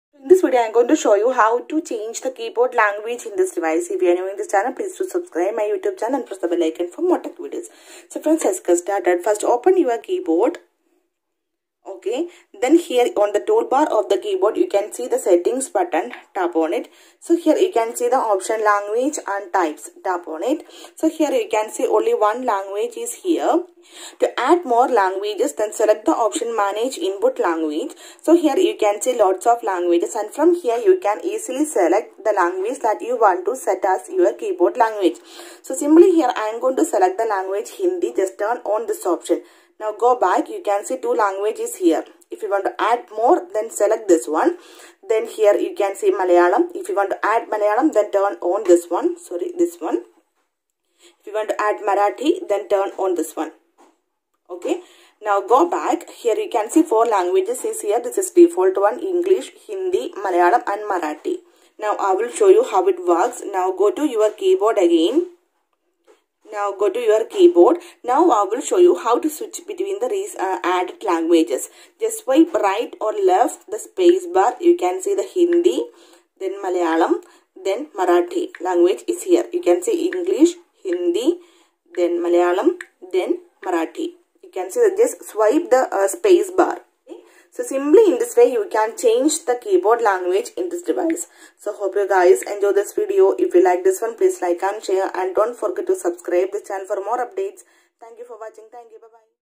Samsung Galaxy M42 5G | How To Change Keyboard Language in Samsung Galaxy M42 5G
Hi friends this video shows you that " How to change keyboard language in Samsung Galaxy M42 5G "